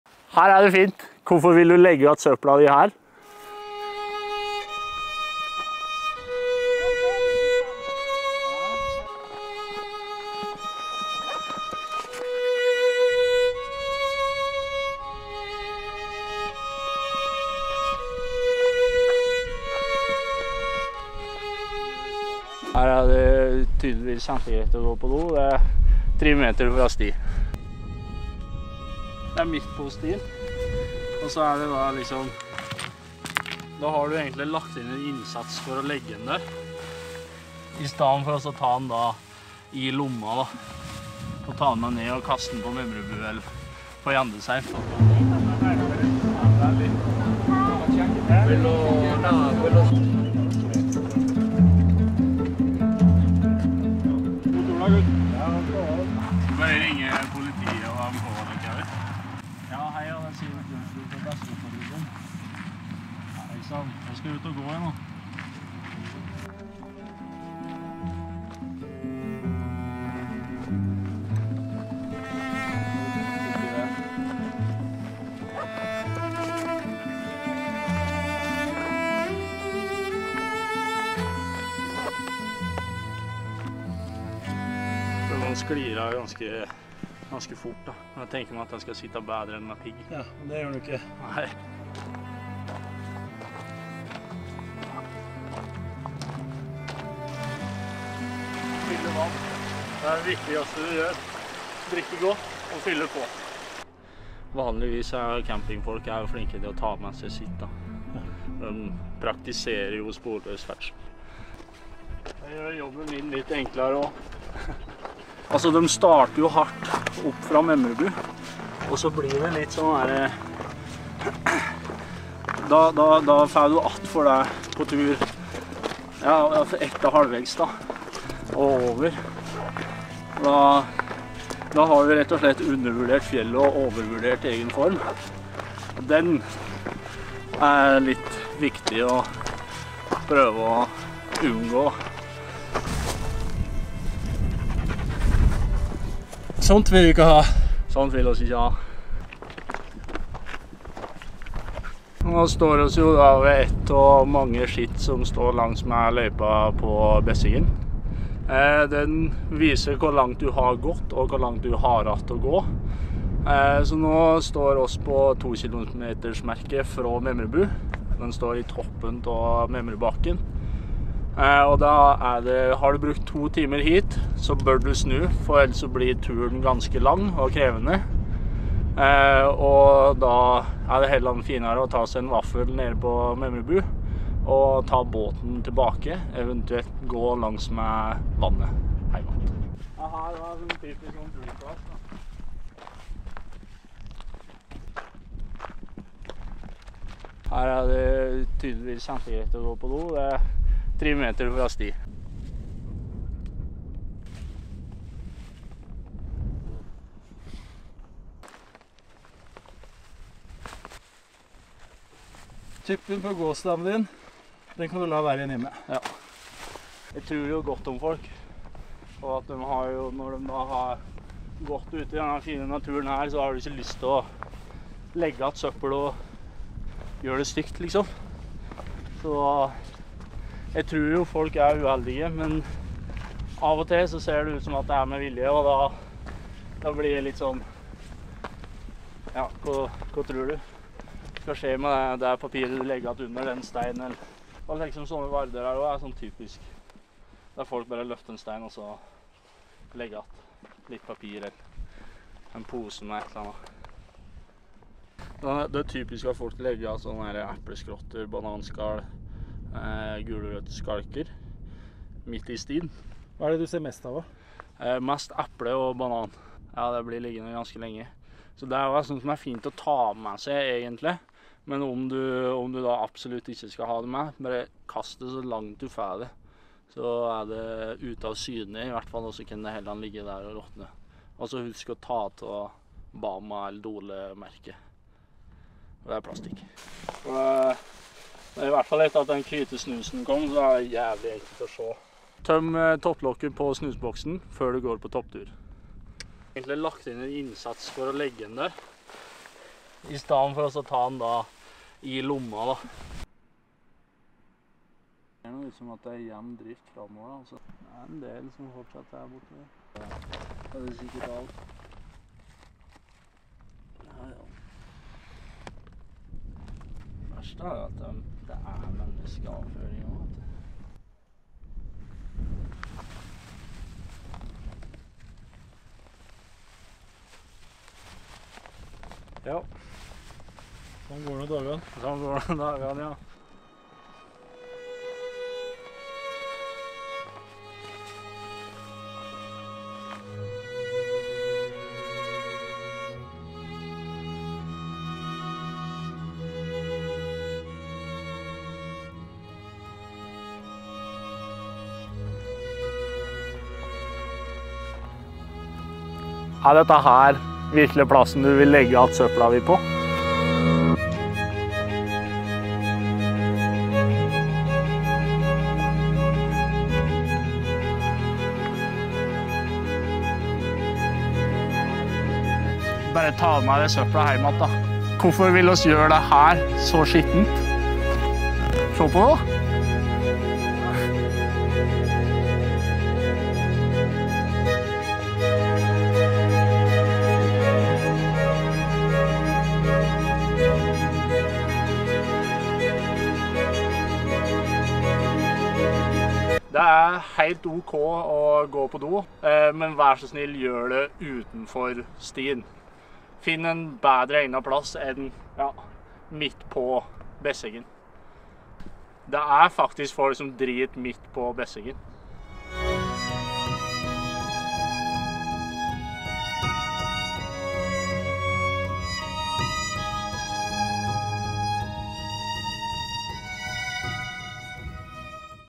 PLUKK SØPPEL MED BESSEGGEN PATRULJA
Hvor mye søppel er det egentlig på Besseggen etter en dag?

Vi ble med Besseggen Patrulja over Besseggen og plukket litt av hvert! Få tips om hva du helst ikke skal gjøre når du går Besseggen i sommer!